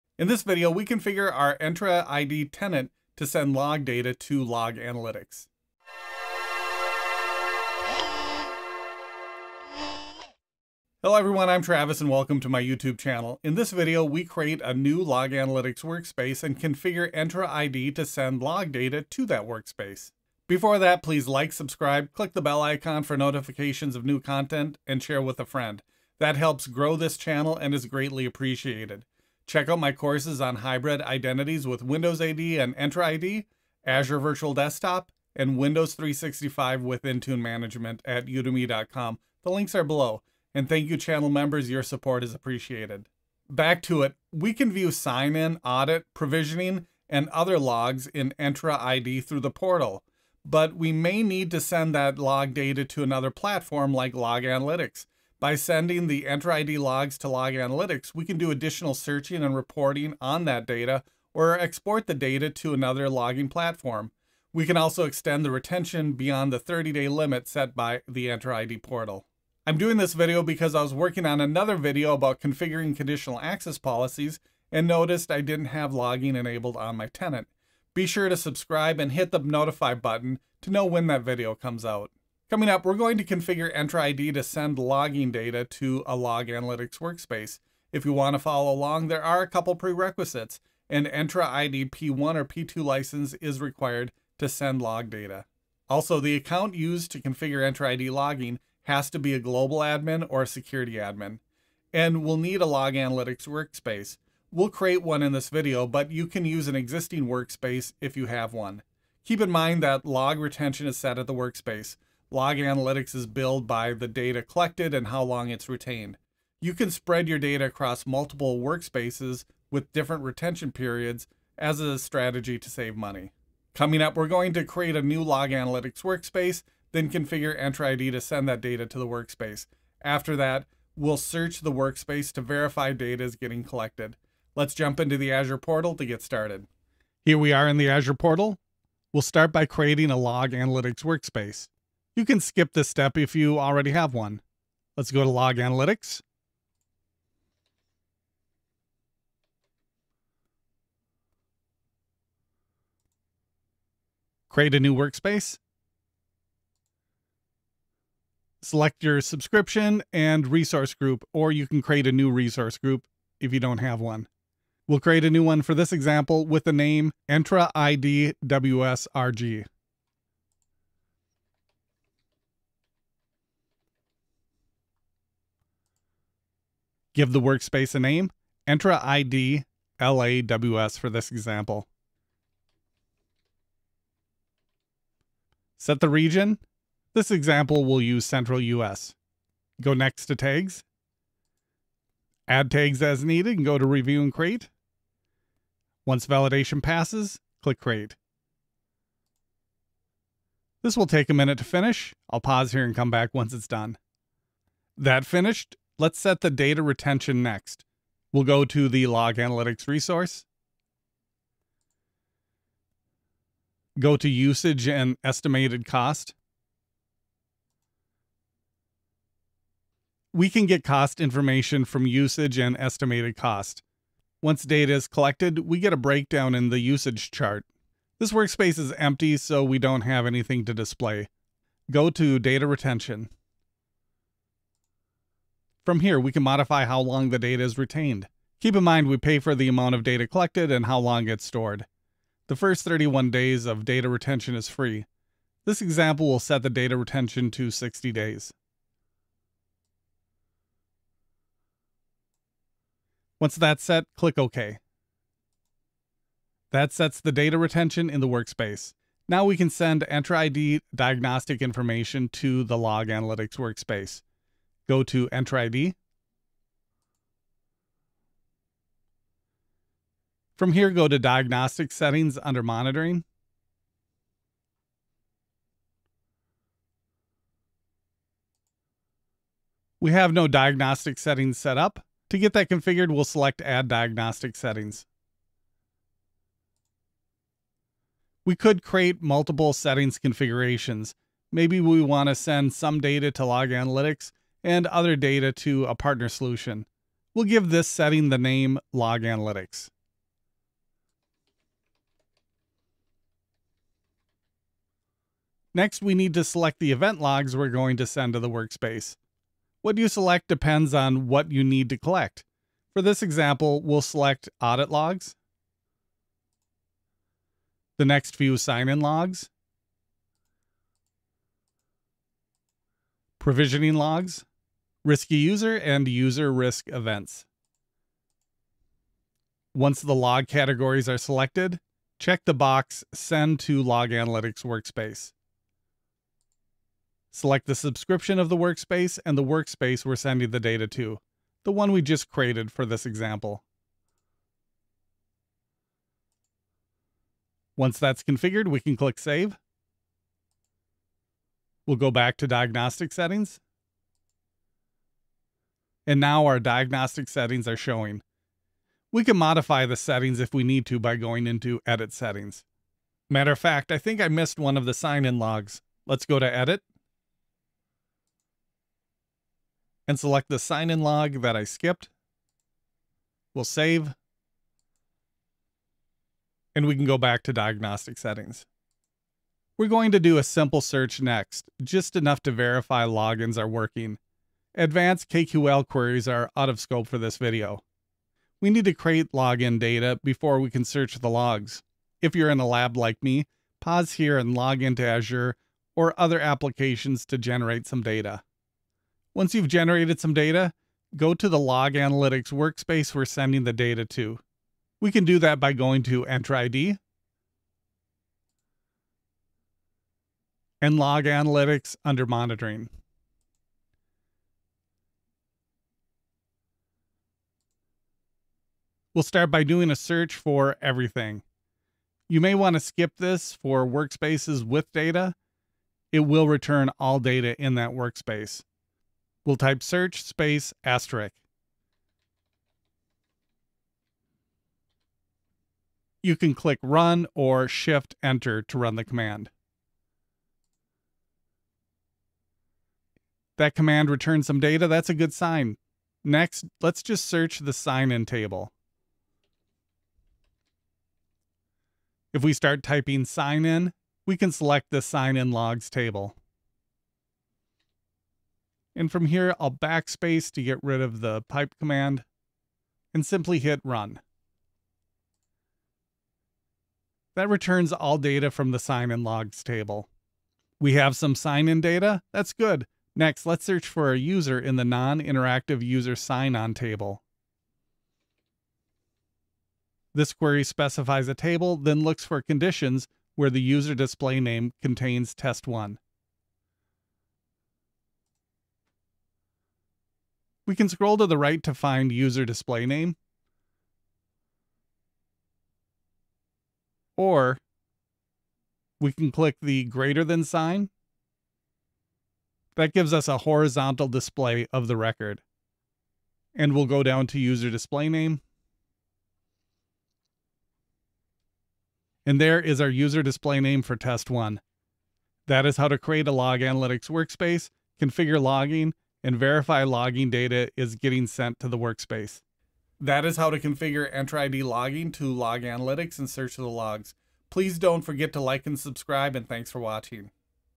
How to Send Entra ID Logs to Log Analytics
We can view Entra ID log data from the portal, but that is only available for 30 days.  We may want to store important log data like sign in activity and risky user actions for more than 30 days.  Or we may need to send that data to other systems for future analysis and storage.  This video goes over how to send Entra ID logs to Log Analytics.  It starts with setting up a new Log Analytics workspace, then configuring logging to the workspace.  Finally, we run queries in Log Analytics to verify logging is enabled.

00:00 - Start
02:17 - Create a Log Analytics Workspace
03:24 - Set Log Analytics Data Retention
04:19 - Send Entra ID Log Data to Log Analytics
06:19 - Search Log Analytics